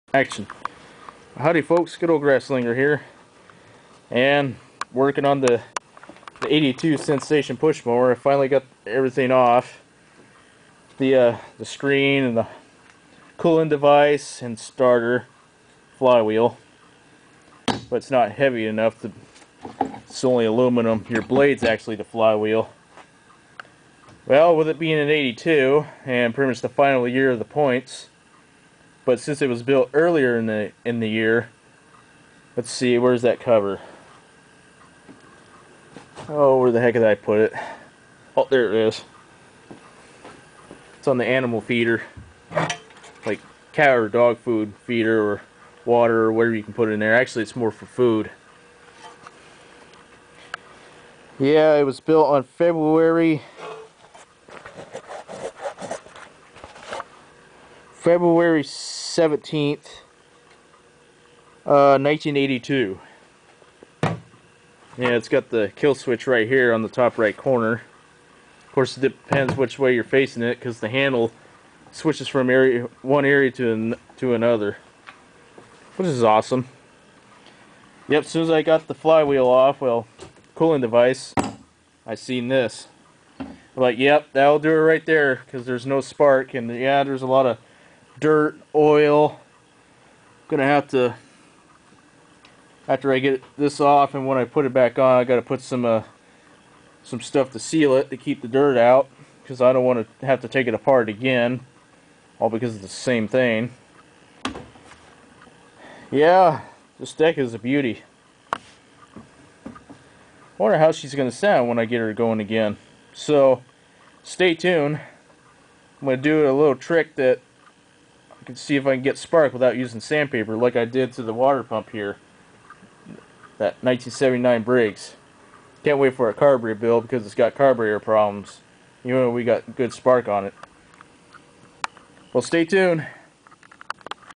Getting the 1982 SENSATION push mower to run #1
Now I got to clean the points because there's no spark. Filmed on 10/29/12.